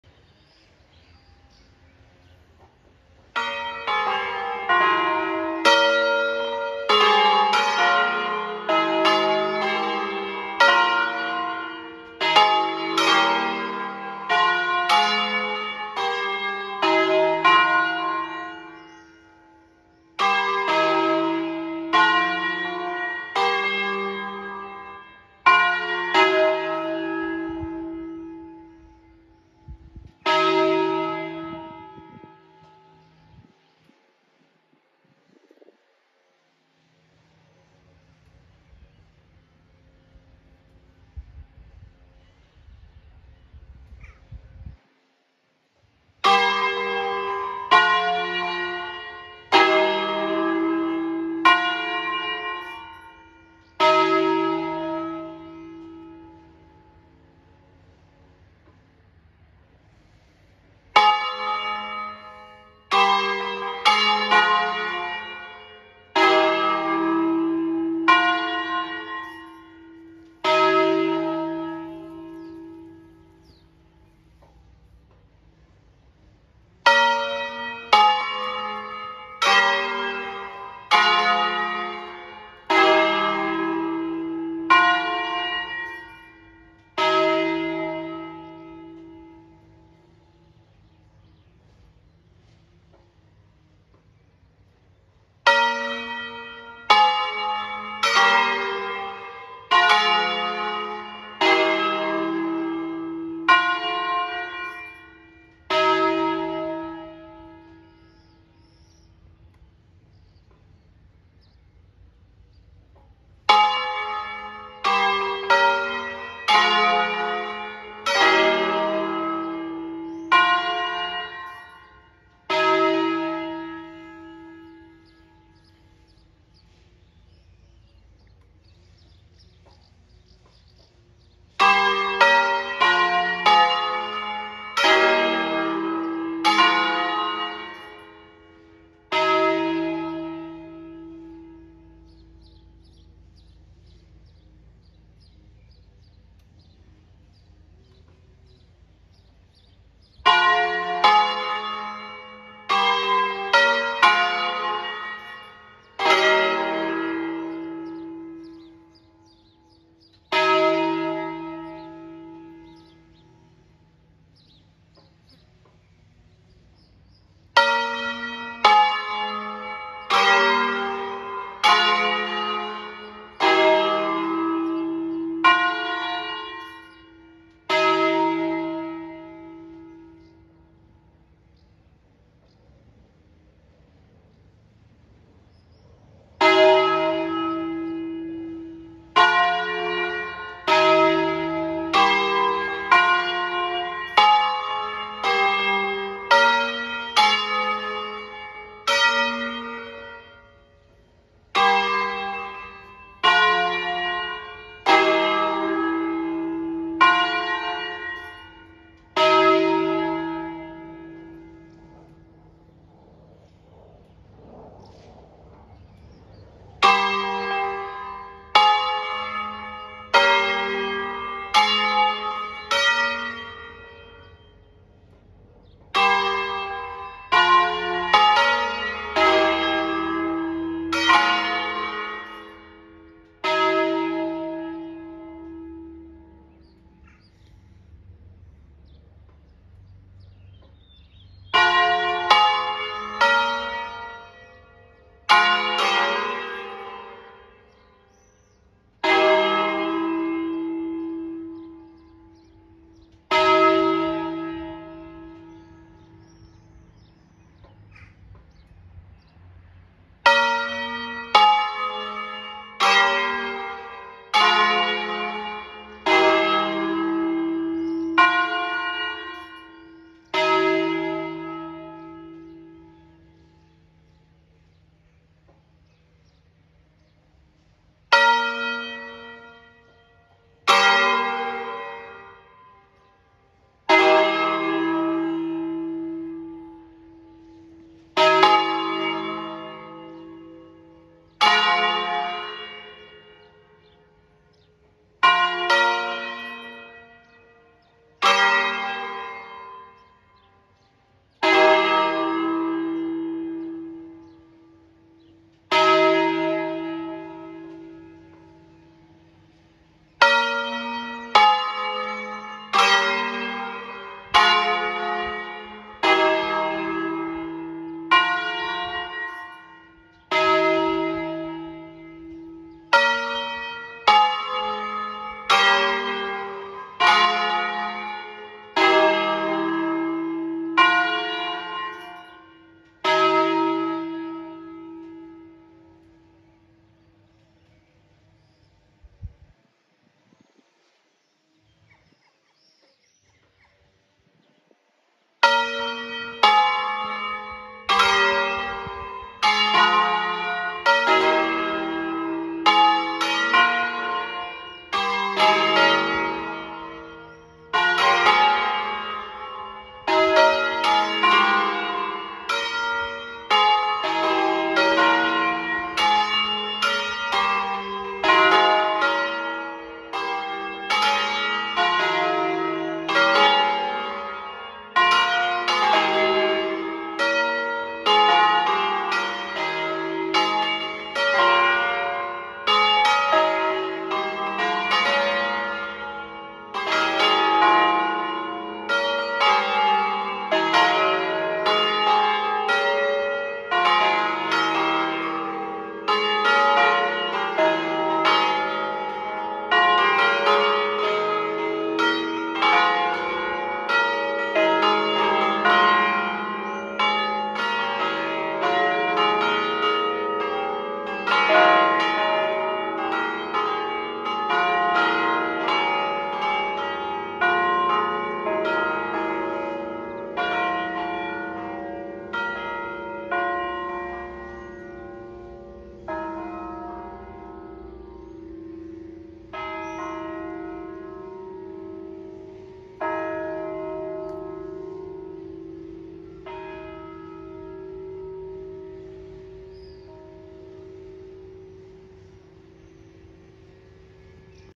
Le campane di Bianzano (BG) - Nuovo concerto solenne corto -
Bianzano (BG), Chiesa Parrocchiale di S. Rocco.

5 campane in Mib3, Crespi 1853 ( Mib3 RIF. D’ Adda  1953 ).

- Concerto solenne corto - a fine manutenzione straordinaria

Interventi eseguiti:

Inserimento nuovo gruppi comando a bordo motore
Sostituzione quadro di potenza
Correzione bilanciature campane n1/2
Ripristino sistema di suono a corda
Revisione battagli 1/2/3
Sostituzione battagli 4/5
Rimozione pesi installati su ruote
Messa in asse campane
Registrazione nuovi concerti
Verniciatura totale della castellatura